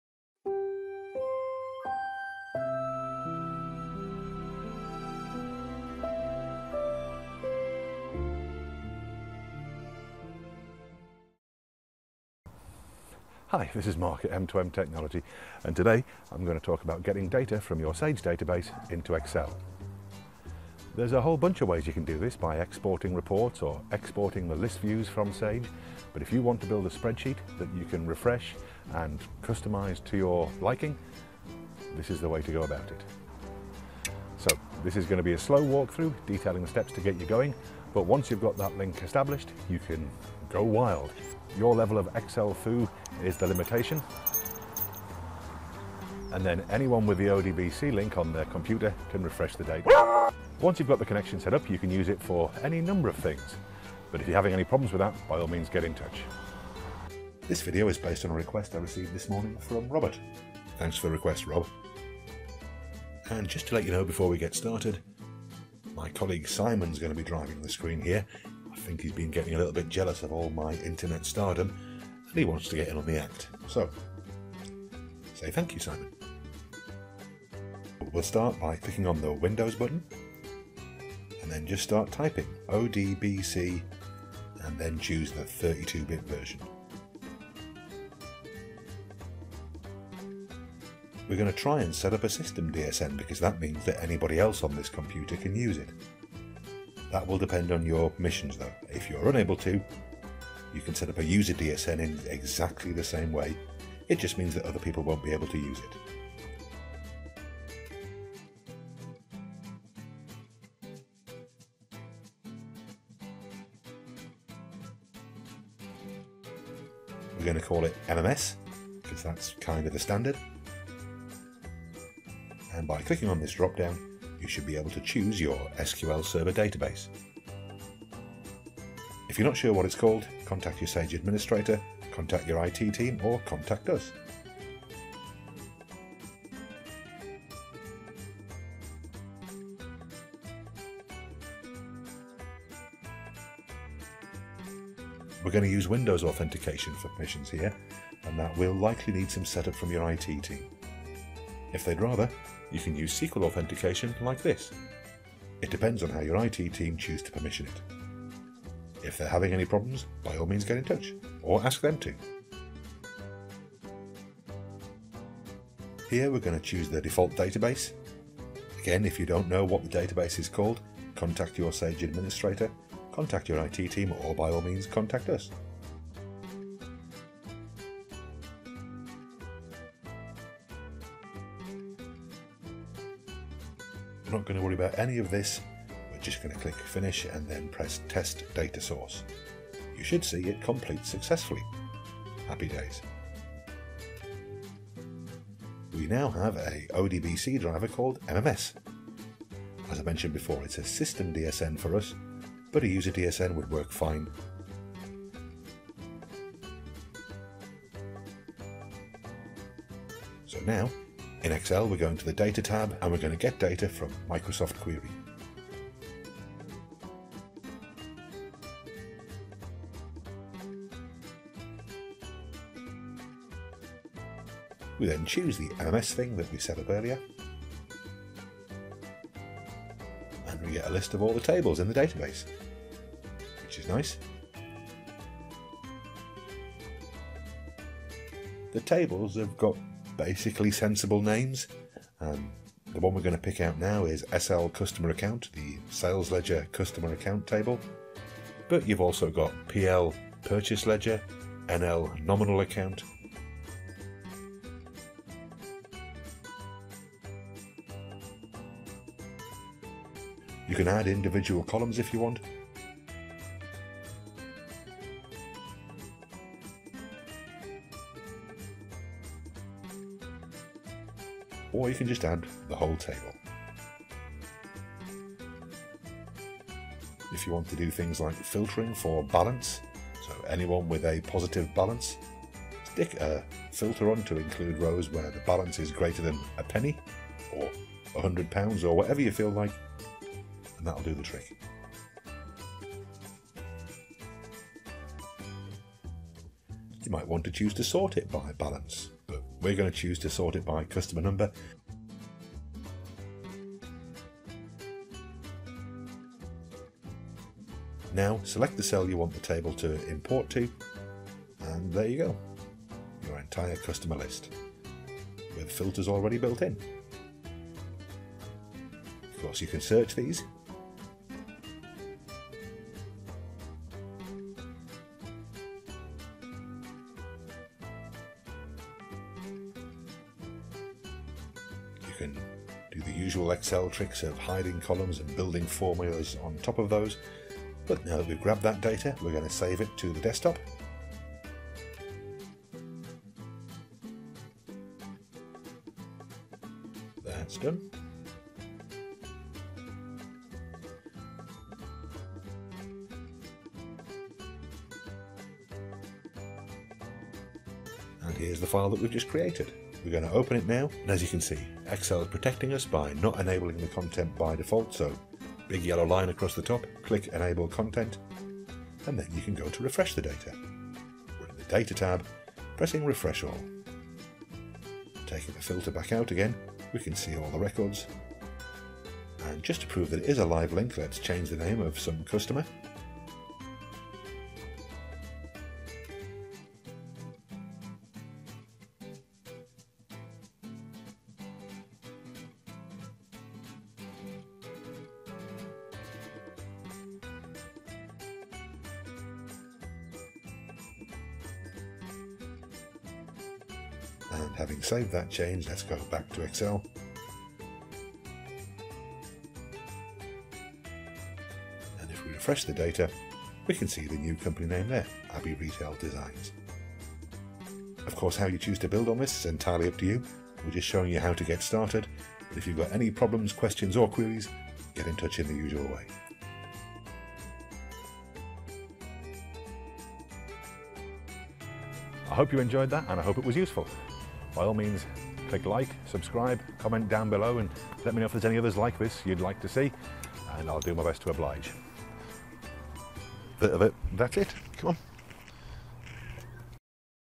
ODBC Setup for Sage 200c - how to link Excel to your SQL Database. From M2M.
Another slower paced video detailing the steps you can follow to setup an ODBC link into your Sage 200c SQL database.

m2mit.uk

support.m2m.host